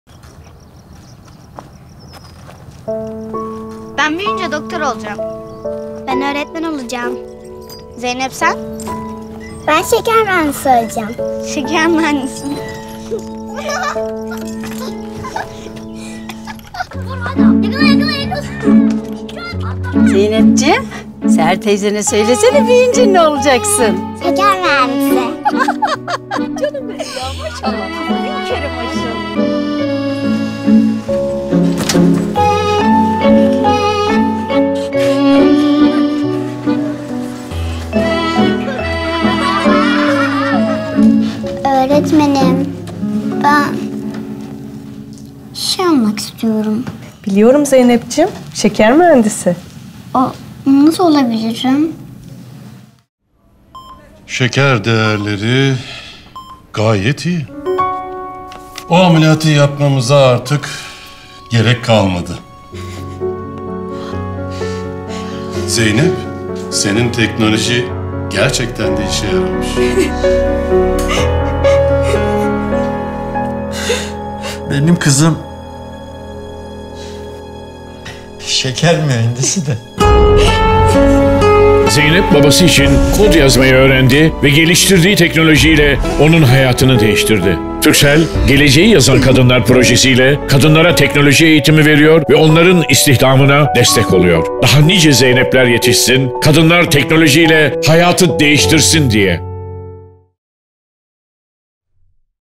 Şeker Mühendisi
"Turkcell Geleceği Yazan Kadınlar" projesi ile kadınlara teknoloji eğitimi veriyor ve onların istihdamına destek oluyoruz. Kadınlar teknoloji ile hayatı değiştirsin diye.